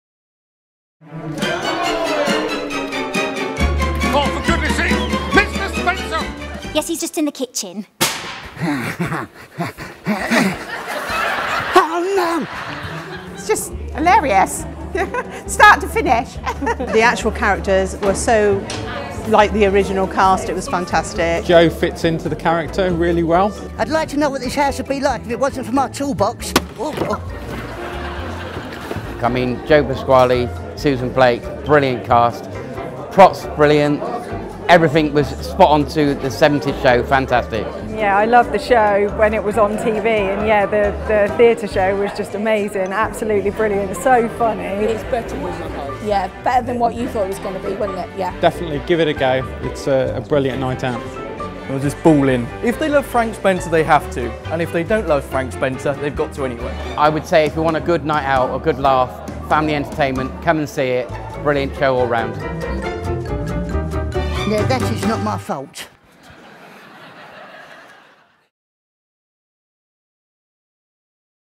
Some Mothers Do 'Ave 'Em | What the audience said...
Tue 17 - Sat 21 Jul

Everyone’s favourite 1970s classic TV comedy, Some Mothers Do ‘Ave ‘Em is back with Joe Pasquale starring as the loveable accident-prone Frank Spencer, Sarah Earnshaw as his wife Betty and Susie Blake as his disapproving Mother-in-Law, Mrs Fisher.

Here the audiences so far tell us what they thought of 'this wonderfully entertaining and hilarious play' (LondonTheatre1)!